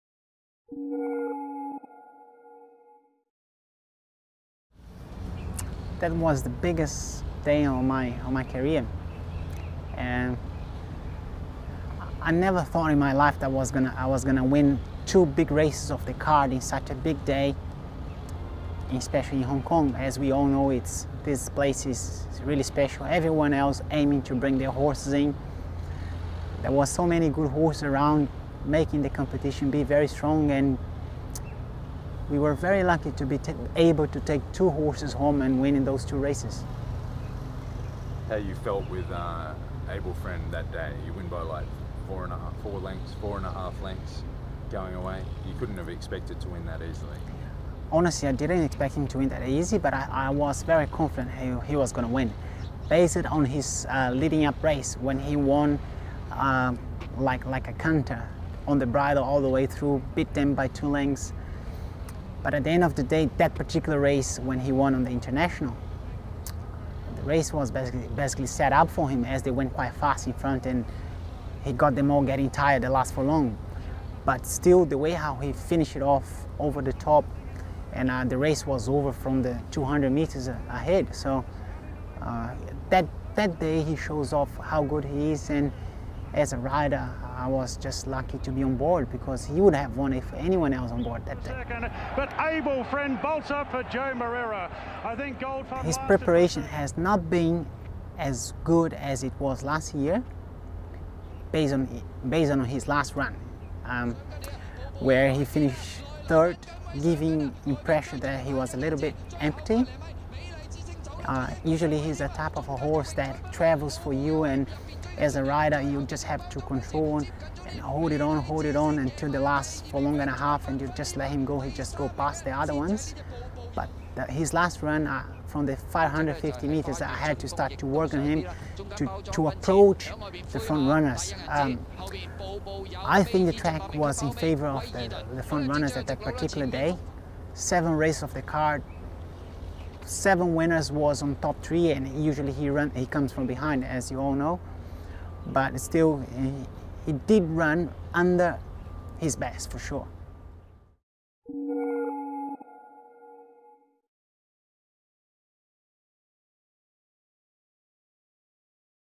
Champion jockey Joao Moreira discusses Able Friend ahead of the 2015 Longines HK International Races
Hong Kong\'s champion rider believes the superstar can bounce back to his best in the Hong Kong Mile.Photo: Kenneth Chan/SCMP

This video was originally published on 8 December 2015